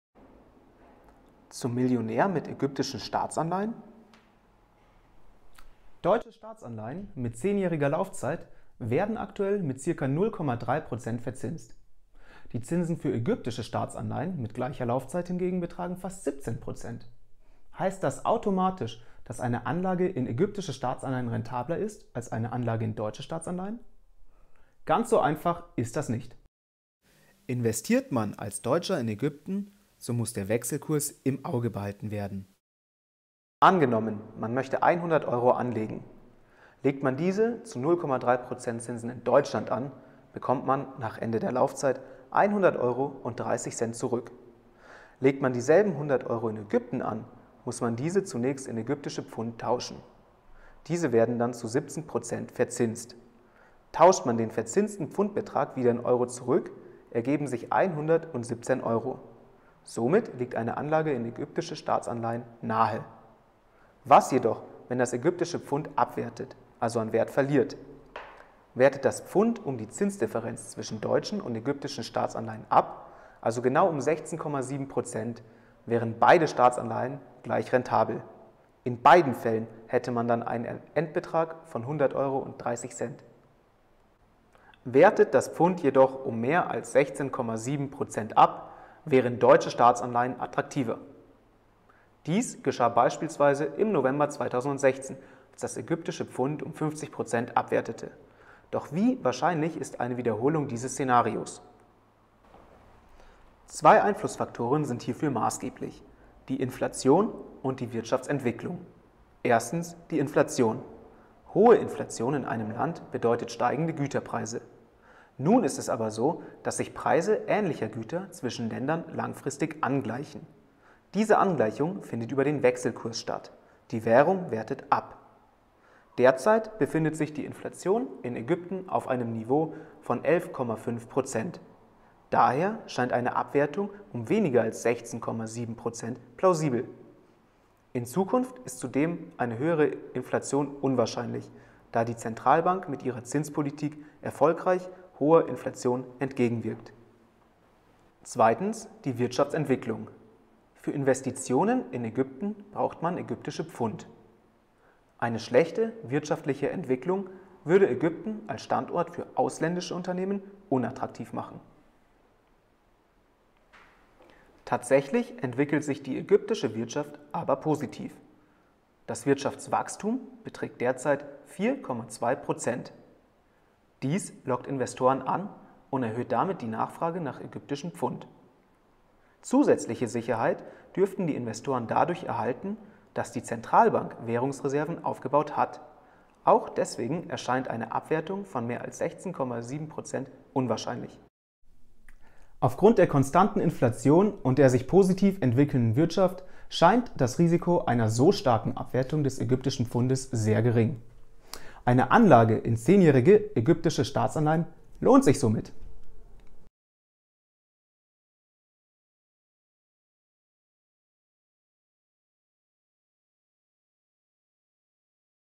Zum Millionär mit ägyptischen Staatsanleihen?
Ägyptische Staatsanleihen locken mit hohen Zinsen. Aber lohnt sich eine solche Anlage angesichts einer möglichen Abwertung des ägyptischen Pfunds? Im Rahmen eines Masterseminars beantworten drei Studierende der Universität Passau diese Frage.